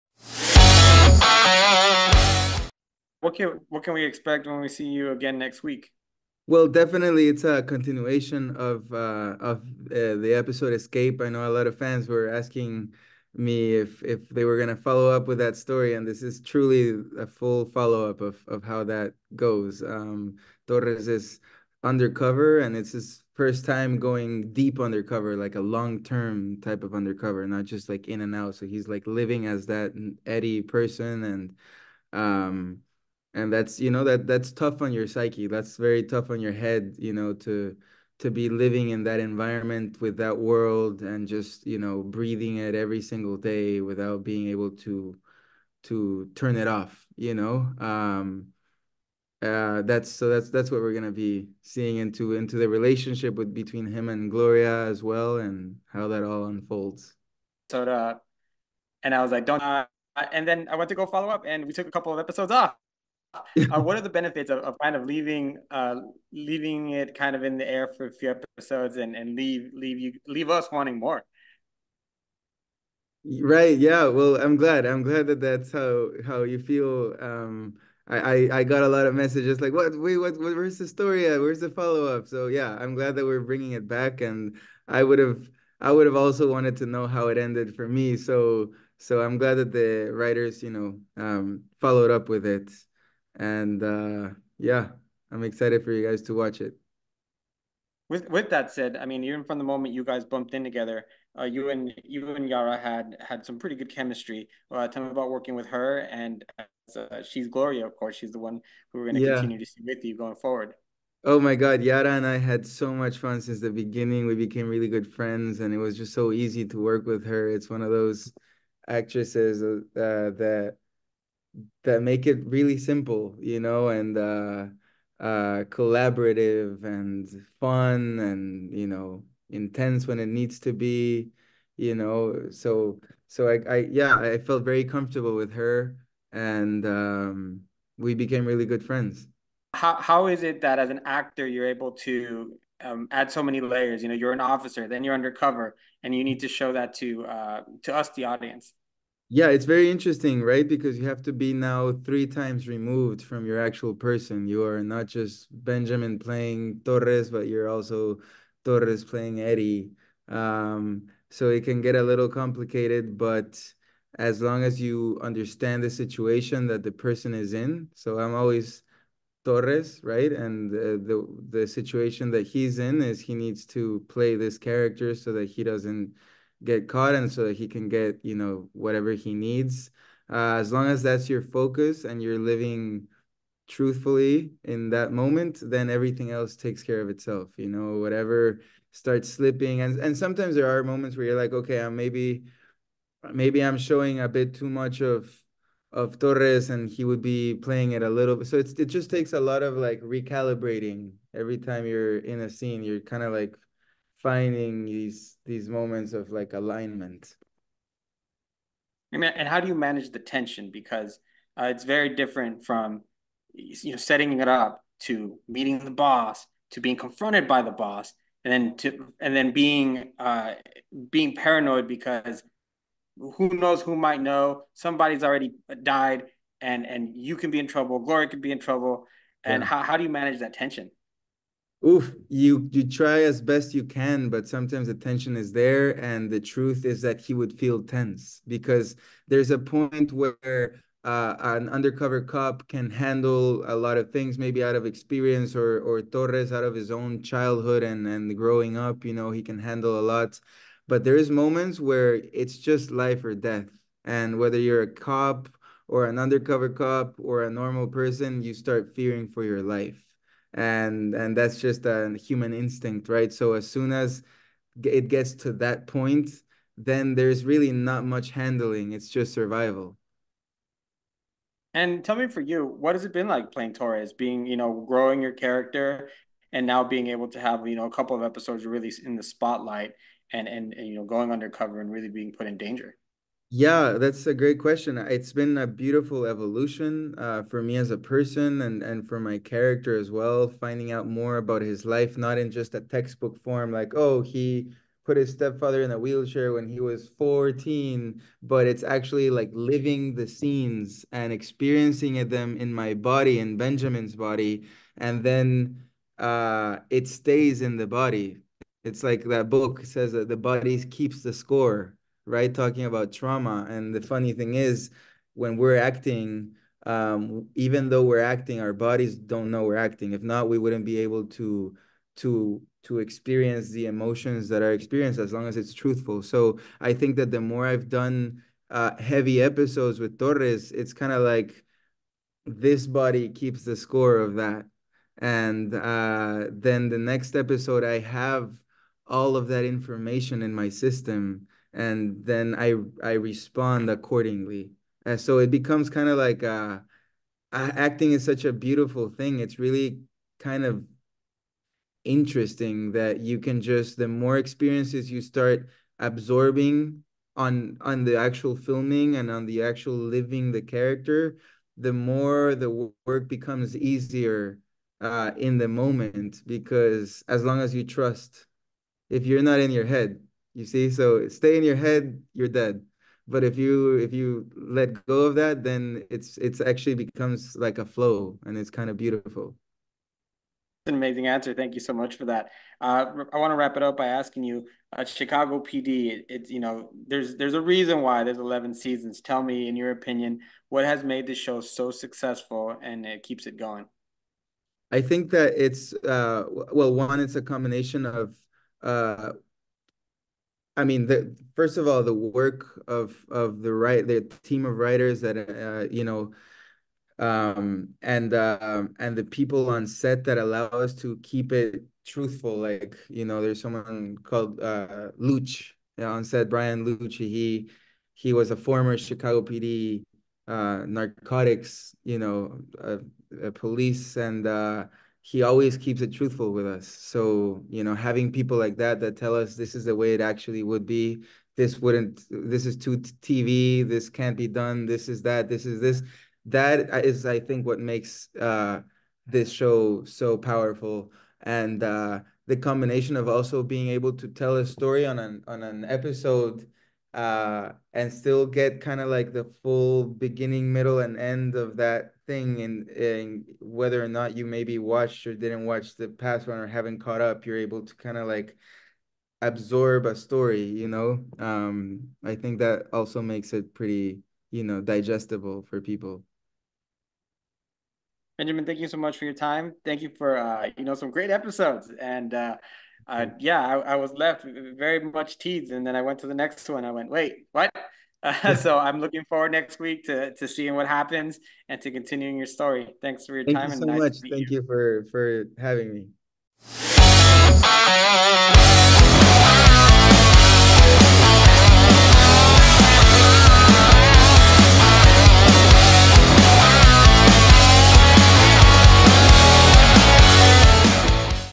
Benjamin Levy Aguilar Excited About New Season of Chicago P.D.
NEWS X MUSE Manny Gomez spoke with Benjamin Levy Aguilar. He is one of the stars of NBC’s “Chicago P.D.” as Dante Torres. His character is fresh off patrol duty and comes into intelligence as a rookie. Torres is the focal point of an undercover storyline in season 11 of the series.

In our conversation with Aguilar, we talked about the cliffhanger that they left us on in episode four of Chicago P.D. 's 11th season titled, “Escape”. He teased the episode and talked about working with Yara Martinez who plays Gloria in the series and a key character in this undercover arc. Aguilar also shares with us his experience playing Torres and how its been as an actor playing a character with many layers. It was a great conversation that you can check out above!

Here is more information about Benjamin Levy Aguilar

Aguilar is best known for his role on "Filthy Rich" and recently recurred on Netflix's "The Woman in the House Across the Street from the Girl in the Window," opposite Kristen Bell.

Born and raised in Guatemala, Aguilar was a standout athlete and excelled in soccer as a child. He was recruited to participate in the junior academies for the world-renowned Italian football club, AC Milan. He later studied Krav Maga and his discipline led to a position with the largest security firm in Guatemala, where he served during the 2011 Guatemalan presidential election. At the age of 18, he decided to pursue acting and moved to Los Angeles where he currently lives.

Here is a synopsis of NBC’s “Chicago P.D.”

From multiple Emmy Award-winning executive producer Dick Wolf and the team behind the hit series "Chicago Fire" and "Chicago Med," "Chicago P.D." is a riveting police drama about the men and women of the Chicago Police Department's elite Intelligence Unit, combatting the city's most heinous offenses – organized crime, drug trafficking, high-profile murders and beyond.

At the center of "Chicago P.D." is Sgt. Hank Voight (Jason Beghe), who is at ground zero against the war on crime in Chicago and is fiercely determined but yet complicated.

His close-knit team of detectives share his passion for keeping the city safe, including Officer Adam Ruzek (Patrick John Flueger), a quick thinker who often leads with his heart; Officer Kim Burgess (Marina Squerciati), a fearless former patrol officer whose research and astute observations often lead the team in the right direction; Officer Kevin Atwater (LaRoyce Hawkins), a bright and quietly charismatic member of the team; and Det. Hailey Upton (Tracy Spiridakos), a seasoned detective whose tough demeanor belies her complicated emotions.

Desk Sgt. Trudy Platt (Amy Morton) runs a tight precinct with a firm hand, although she lets her wry sense of humor shine through from time to time.

The Intelligence Unit will be tested as it learns how to work efficiently with a new chief of police. In addition, a new member has been added to its ranks – Officer Dante Torres (Benjamin Levy Aguilar), who is fresh off patrol but will hold his own on a team with big personalities.

Host:
Manny Gomez